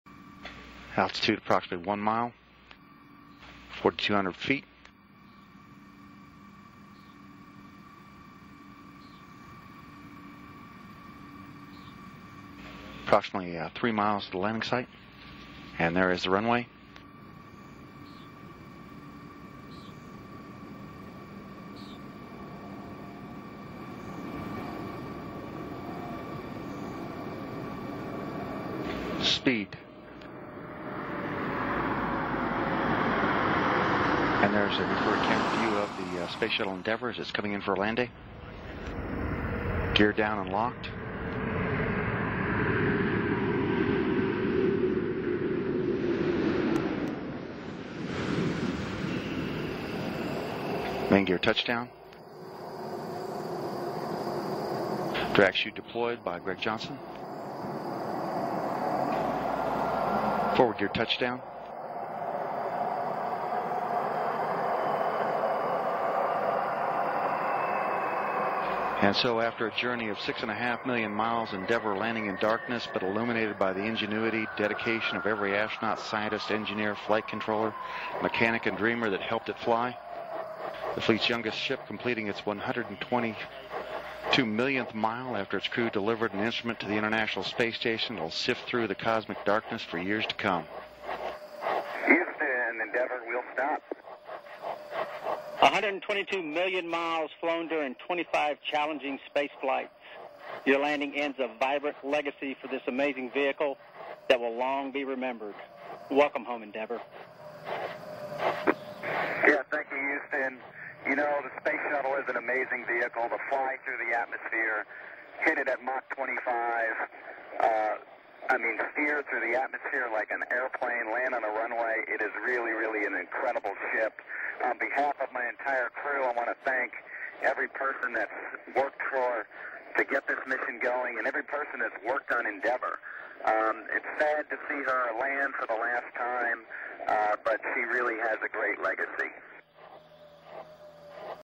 Space Shuttle Endeavour's Final Touchdown
The STS-134 crew was safely returned to the Kennedy Space Center at 2:35am EDT on June 1, 2011, concluding the storied career of this fantastic piece of machinery,